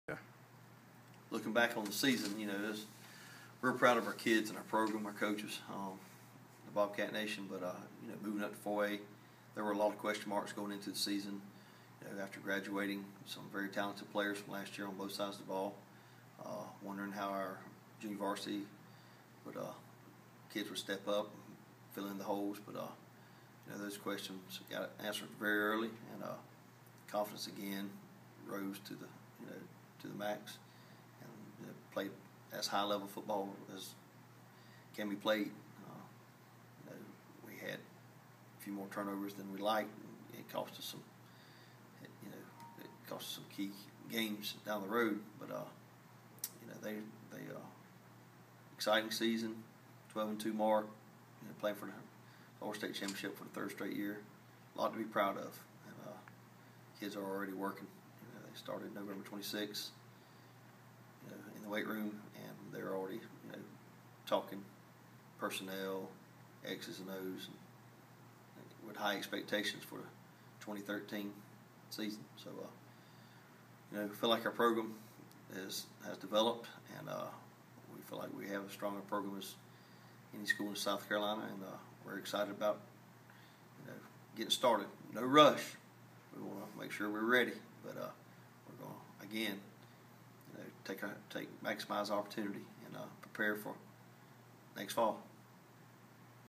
Bluffton football wrapup
Bluffton coach Ken Cribb sums up the 2012 season.